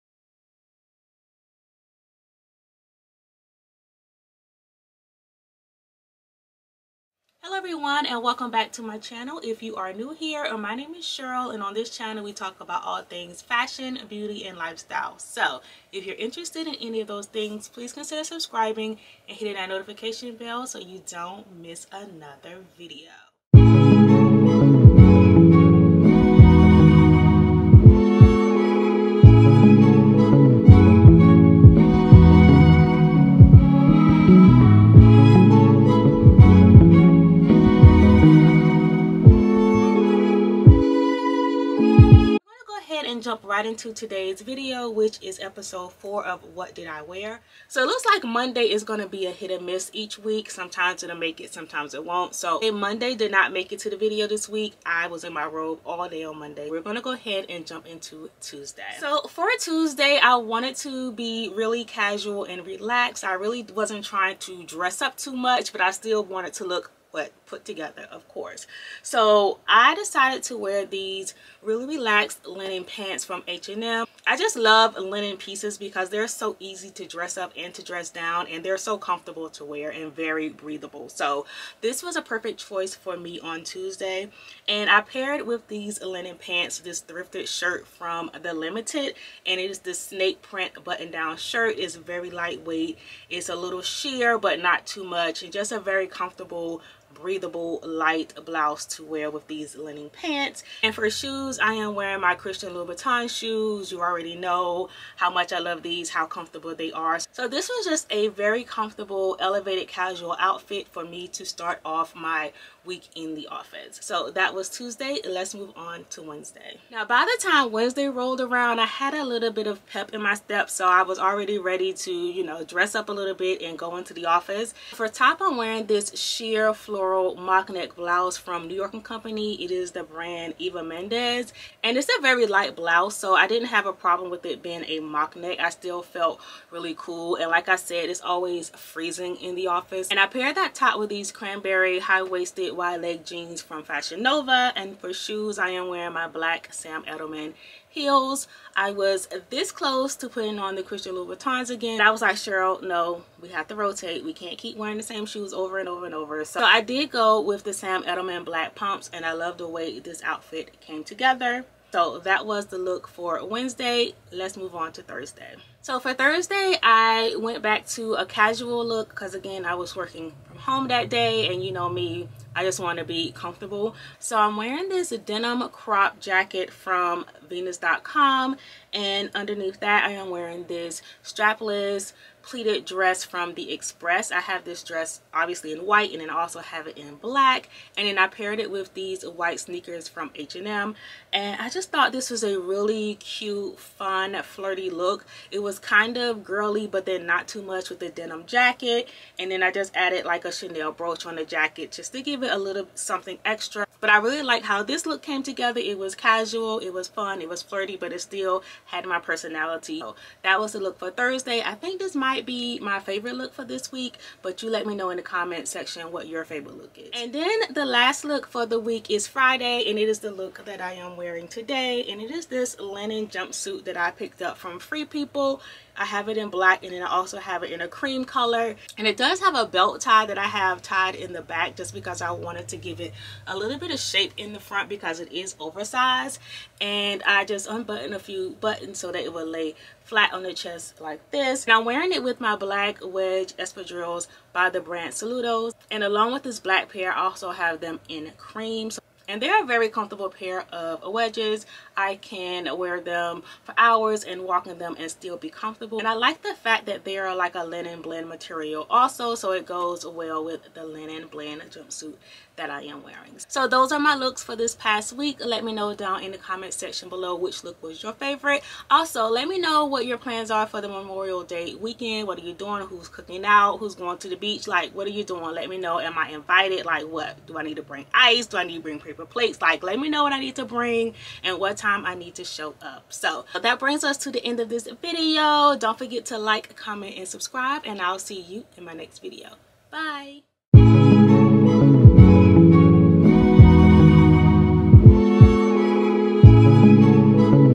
WHAT I WORE THIS WEEK: EPISODE 4
Welcome back for another episode of "What I Wore This Week".

Lemon8: @cheryllyniece